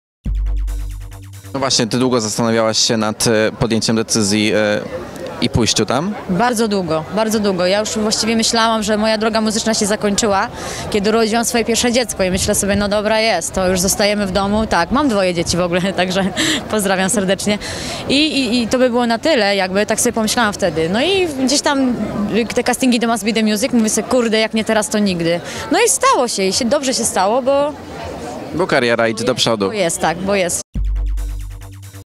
Justyna Sawicka: Kariera w "Must be the Music" po urodzeniu dziecka jest niemożliwa?
Pokaz Patrycji Kujawy - 12.09.17 r.